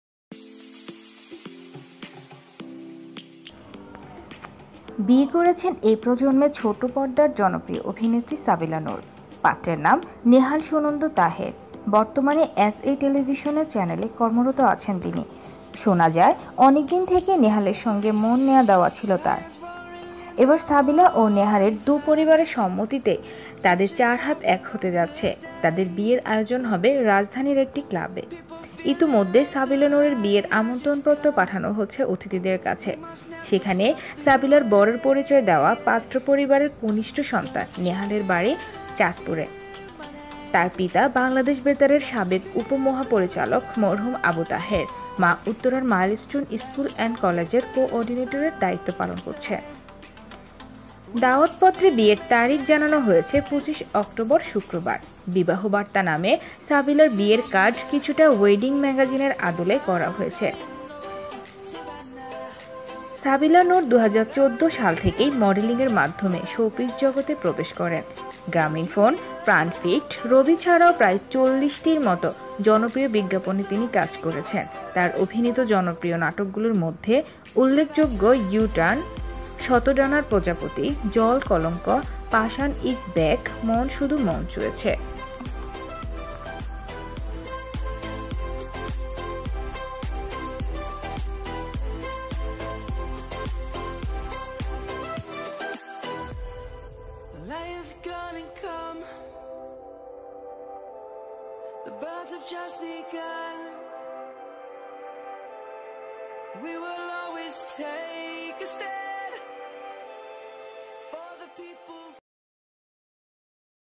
সাবিলা নুর এর বিয়ে || বিয়ে করছেন সাবিলা নুর || Sabila Nur Wedding Pictures
সাবিলা নুর এর বিয়ে||বিয়ে করছেন সাবিলা নুর||Sabila Nur Wedding
 সাবিলার নুর বিয়ের পিরিতে বসতে যাচ্ছেন
সাবিলার বিয়ে
Sabila Nur is a model and an actress
All picture collect are Sabila nur facebook wall,google, Instagram

All Songs Are Used From পায়েল শরকারNocopyright Music [Ncs]